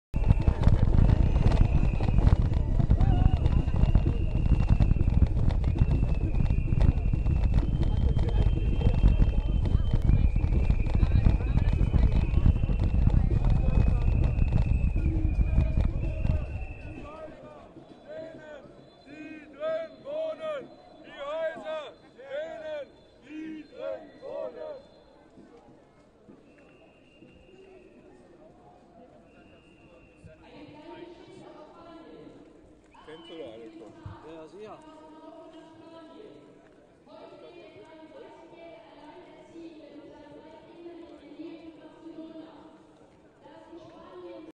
Zwangsraeumung verhindern 08032013 part010
Warm-Up-Demo vor der Zwangsräumungs-Blockade
2. Februar 2013
Sa. // 09.02. // 14 Uhr // Kottbusser Tor
Am Samstag den 09.02. wollen wir gemeinsam unsere Wut über hohe Mieten, Verdrängung und Zwangsräumung auf die Straße tragen. Mit einer Demo durch den Reichenberger Kiez wollen wir Nachbarinnen und Nachbarn informieren und zur Blockade des dritten Zwangsräumungsversuches in der Lausitzer Straße mobilisieren.

Verdrängung hat viele Gesichter: Modernisierung, hohe Mieten, Mieterhöhung nach Mietspiegel, Jobcenter zahlt die Miete nicht, Umwandlung in Eigentumswohnungen. Zwangsräumung ist nur die gewalttätigste Art der Verdrängung.

Das alles ist aber kein Naturgesetz, sondern wird von Menschen gemacht. Und kann deshalb auch von Menschen mit kollektivem und solidarischem Handeln geändert werden. Die verhinderten Zwangsräumungen am Kotti und in der Lübbener Straße haben gezeigt, dass wir erfolgreich Widerstand leisten können. Eine Stadt für alle Menschen kann gegen die Interessen der Profiteure erkämpft werden. Deshalb alle zusammen raus auf die Straße!
- Zur Demo am Sa., 09.02., 14 Uhr, Kottbusser Tor
- Zur Blockade der Zwangsräumung am Do., 14.02., 7 Uhr, Lausitzer Str. 8